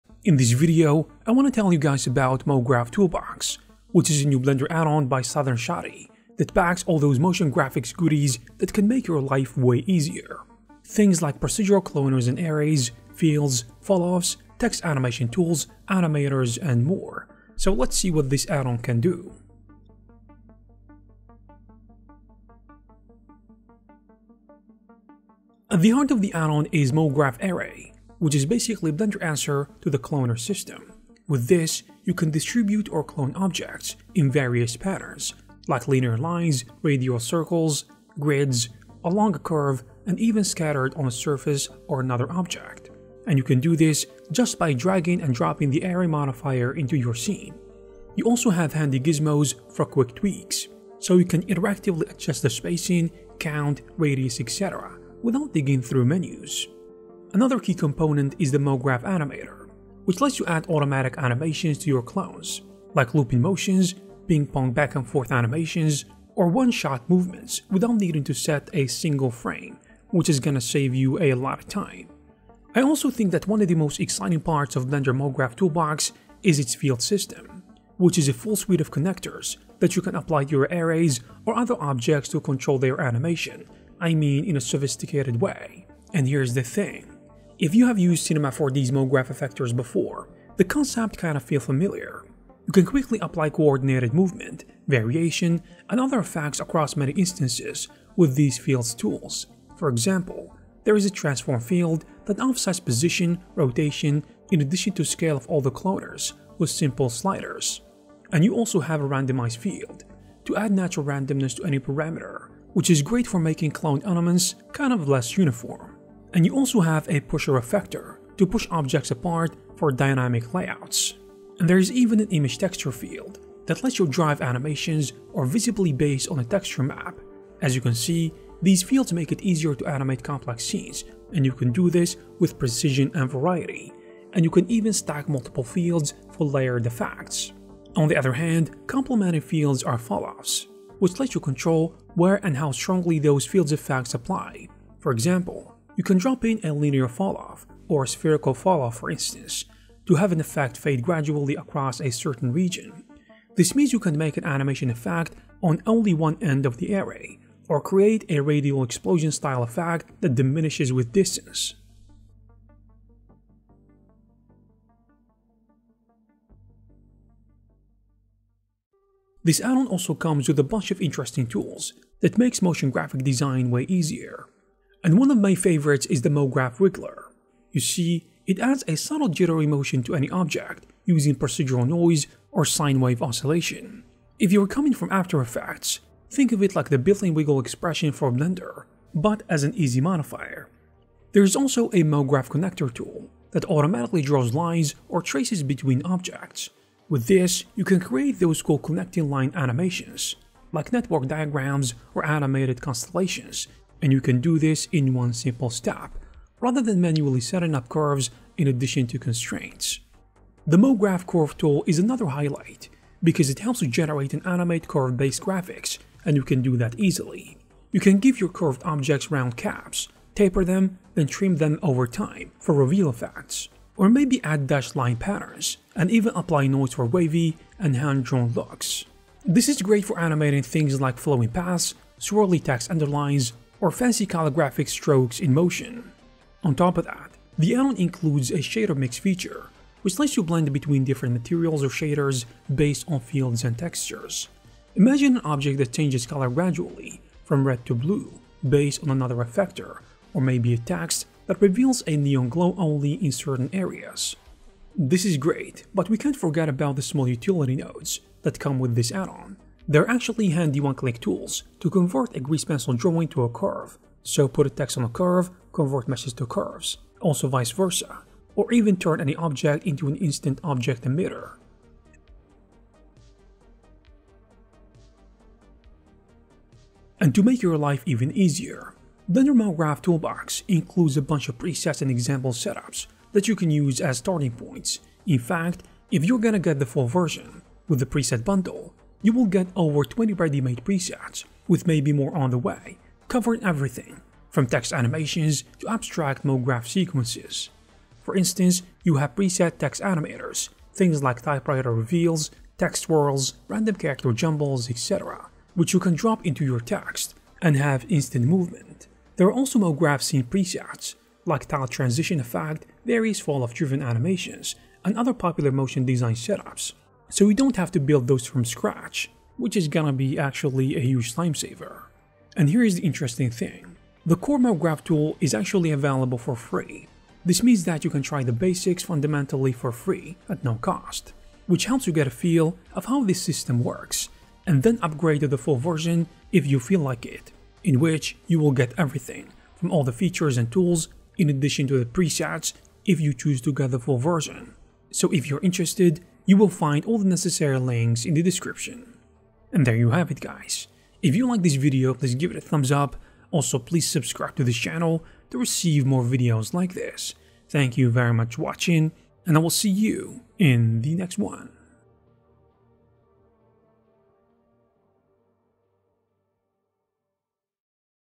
New Blender Addon For Motion Graphics | Mograph Toolbox
Discover the new Blender MoGraph Toolbox add-on by SouthernShotty — a powerful motion graphics toolkit for Blender users.
From procedural cloners to fields, falloffs, text animation tools, wigglers, tracers, and more, this addon makes motion design fast and easy.
If you want to boost your Blender workflow and create pro MoGraph animations, this video shows you everything it can do.